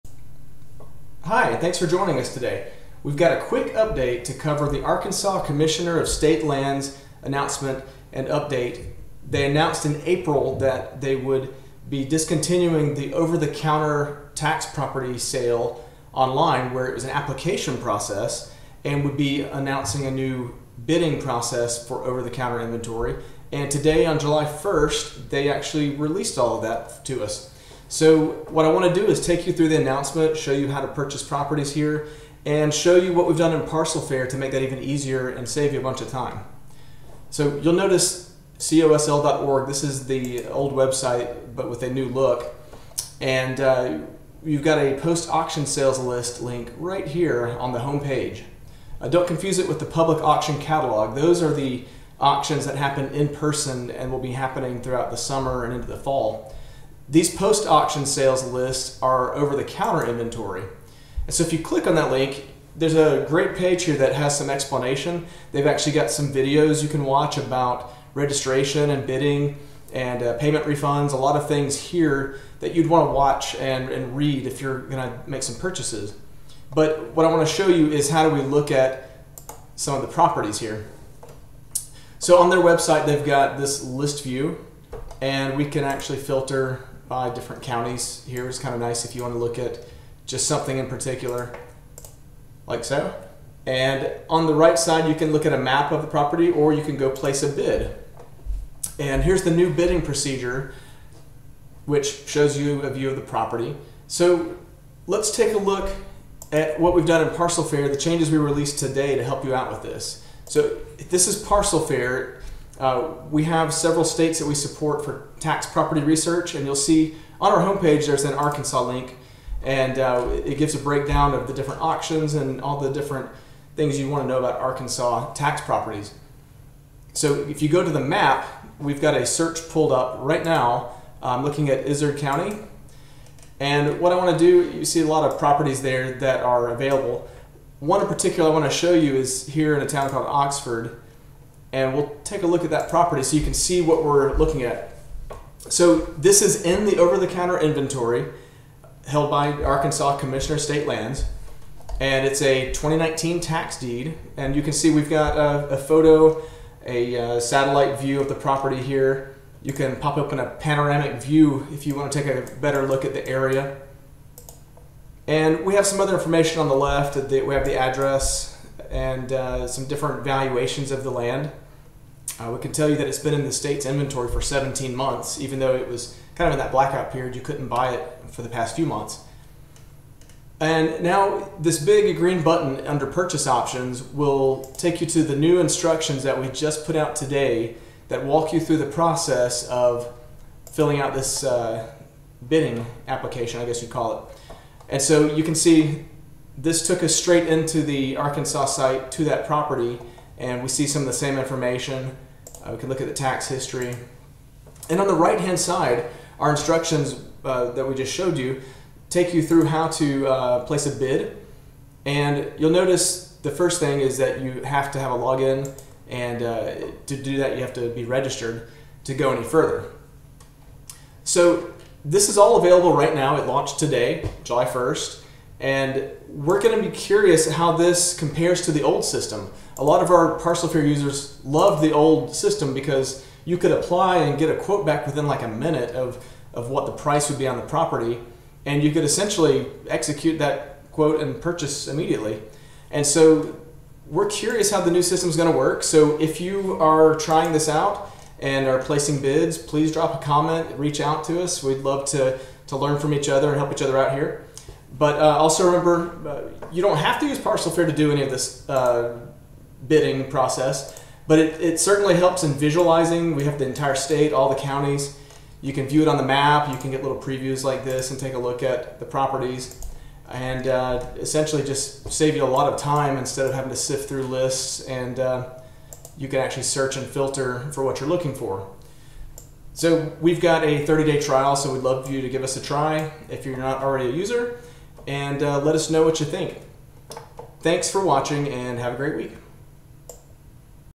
The New Arkansas OTC Tax Property Bidding System (Jul 2021)
The Arkansas Commissioner of State Lands released a new bidding system today (7/1/2021) for over-the-counter tax deed purchasing. The new system initiates an online auction for each OTC property where other investors can bid on the property auction as well. This video shows you how to use the new system as well as how to find and research these properties in Parcel Fair to save hours of time.

to find out more and signup for a free 7-day trial to use Parcel Fair